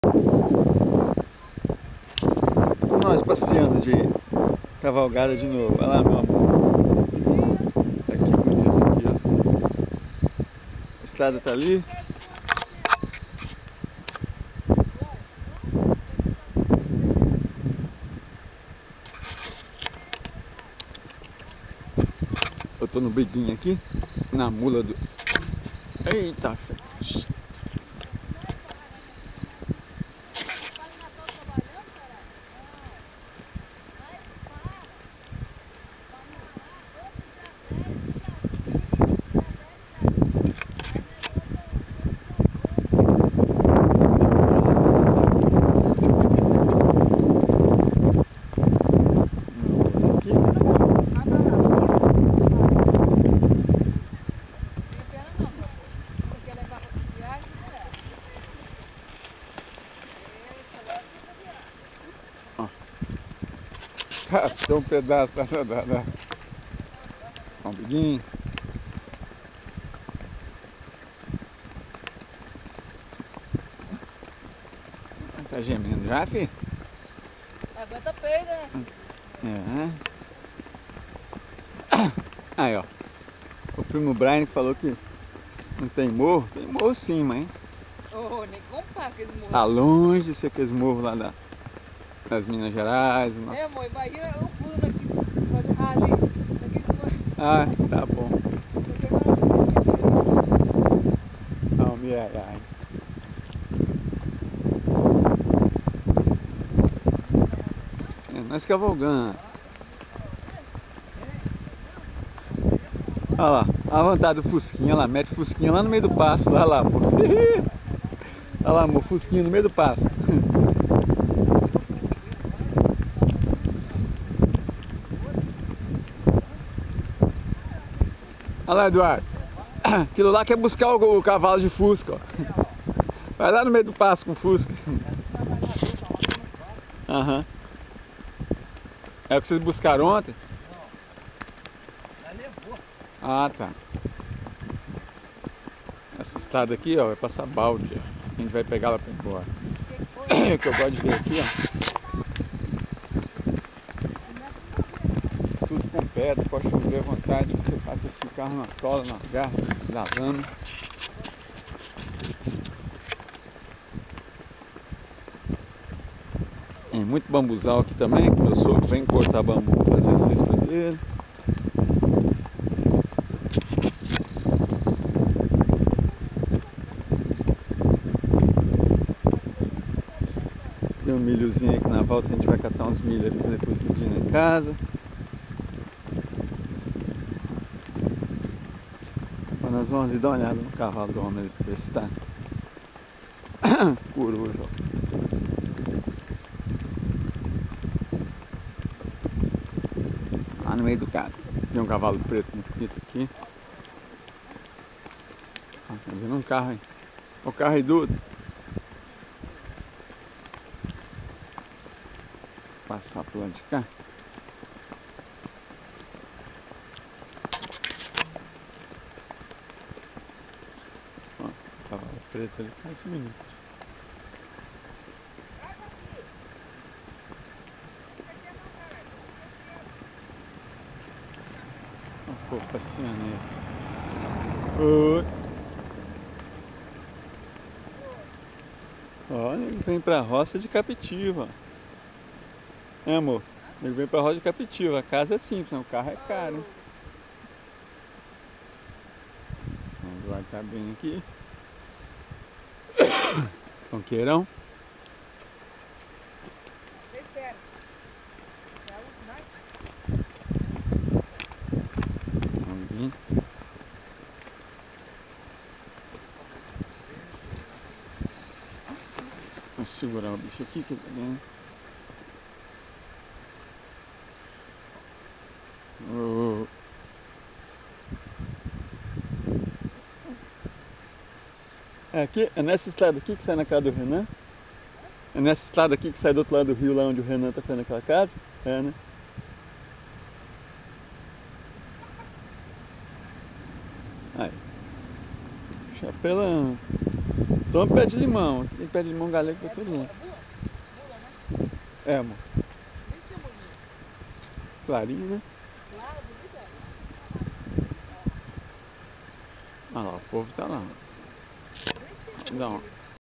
Cavalgada PAU´DALHO - Ep. 141
28/08/2017 - No último domingo com a família na roça, fizemos uma cavalgada nos arredores como despedida. Novamente muito obrigado a todos pela acolhida e carinho. Estamos levando muitas recordações belíssimas de voces. ABRAÇO FORTE E CARINHOSO  a TODOS.